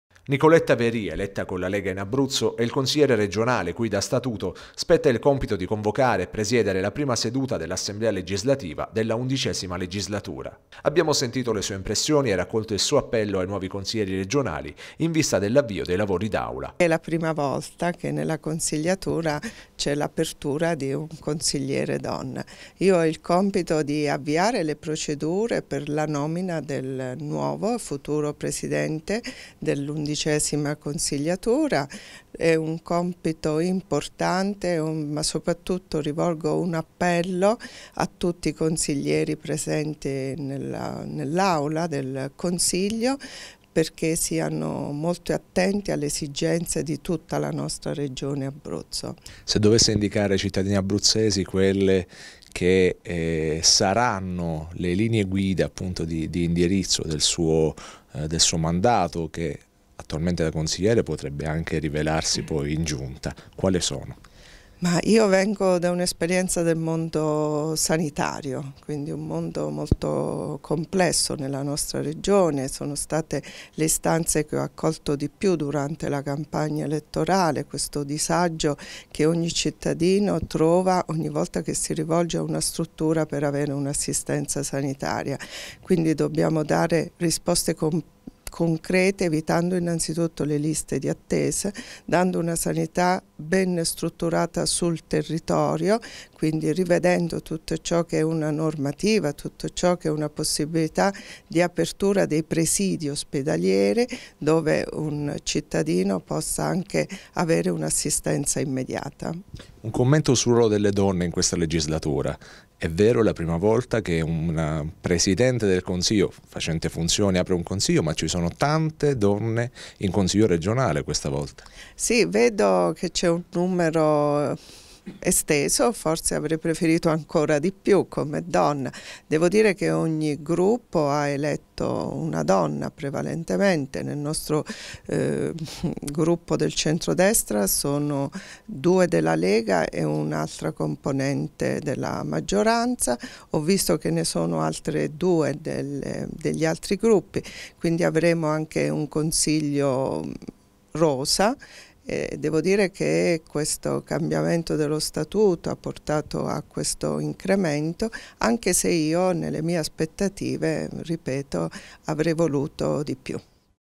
Nicoletta Verì: "I nuovi consiglieri siano molto attenti alle esigenze dell'Abruzzo"
Nicoletta Verì, eletta con la Lega in Abruzzo, è il consigliere regionale cui, da statuto, spetta il compito di convocare e presiedere la prima seduta dell'Assemblea legislativa della Undicesima Legislatura. Abbiamo sentito le sue impressioni e raccolto il suo appello ai nuovi consiglieri regionali in vista dell'avvio dei lavori d'aula.